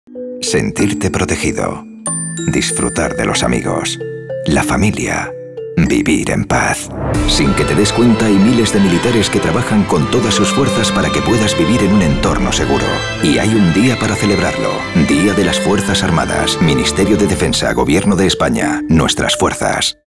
Spot DIFAS 2013 - Alcandora
"Las fuerzas armadas trabajan para que vivamos en un entorno seguro", nuevo anuncio del Ministerio de Defensa para el Día de las Fuerzas Armadas.

El mensaje del Ministerio reconoce la labor invisible de miles de profesionales que trabajan con todas sus fuerzas para que los españoles vivamos en paz.

"Sentirte protegido, disfrutar de los amigos, la familia, vivir en paz... sin que te des cuenta hay miles de militares que trabajan con todas sus fuerzas para que puedas vivir en un entorno seguro", así reza el spot que invita a celebrar el Día de las Fuerzas Armadas.

La campaña ha sido ideada por la agencia independiente Alcandora Publicidad basándose en destacar aspectos que cotidianamente pasan desapercibidos. Buen ejemplo de ello lo representan los miles de militares que en estos momentos se encuentran vigilando el espacio aéreo nacional para garantizar que la defensa del entorno seguro en el que vivimos sea una realidad. Disponemos de un colectivo que hace multitud de cometidos. Todos ellos, favorecen que nuestra sociedad avance y las personas que en ella viven puedan desenvolverse con tranquilidad y certidumbre.
Con esta producción la agencia de publicidad persevera en su apuesta decidida por una comunicación de proximidad, clara, sencilla y --ante todo- muy humana.